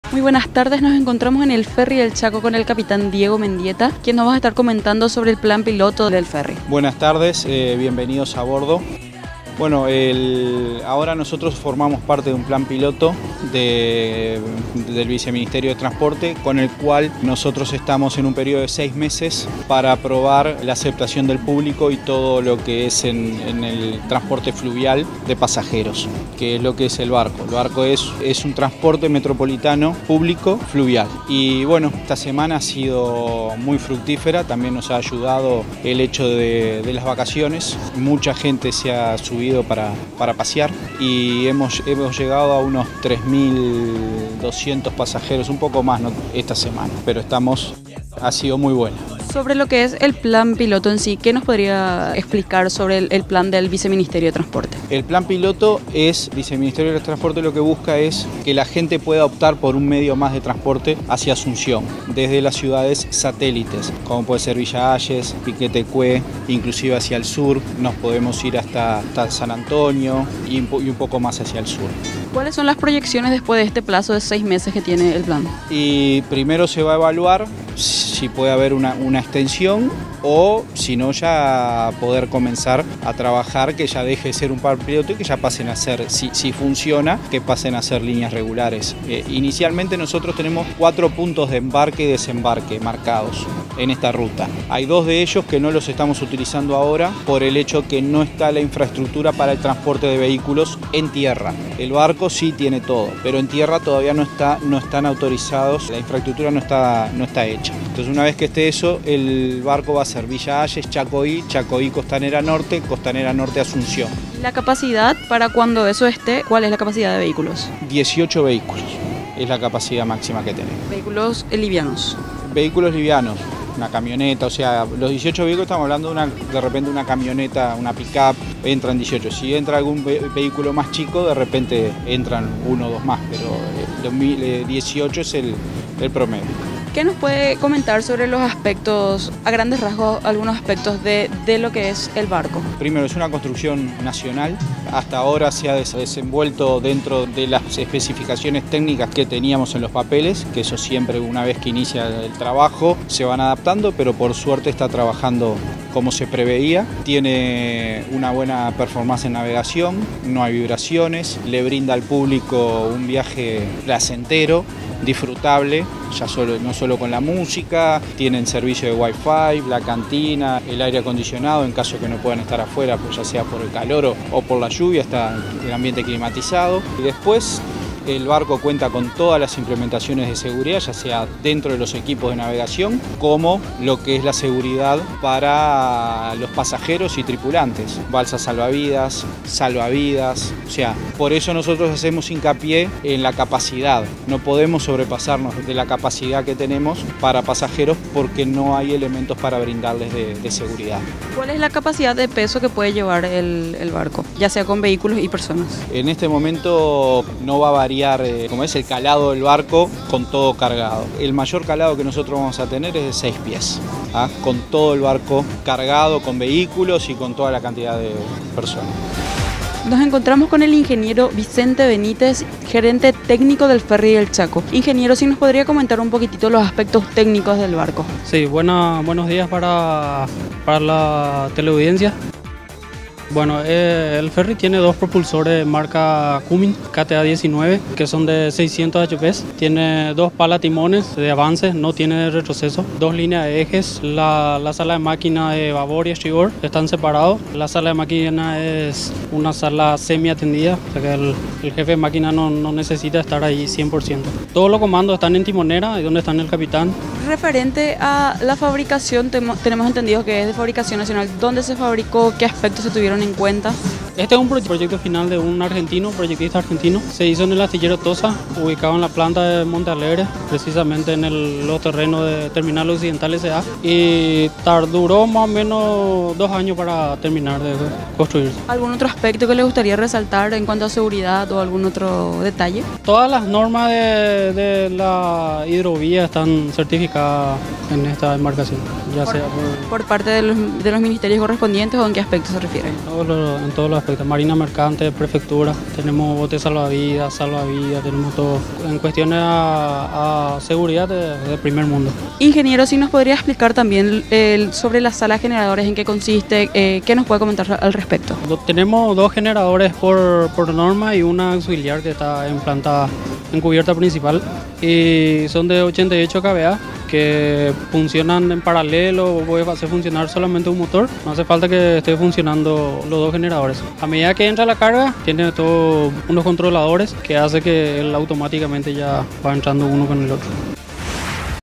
Ferry del Chaco apunta a incrementar más terminales luego del periodo de prueba
En enero, el Ferry del Chaco inició la prueba piloto que durará 6 meses,  para la aceptación de este medio de transporte entre Asunción y Villa  Hayes, explicó el Capitán Diego Mendieta.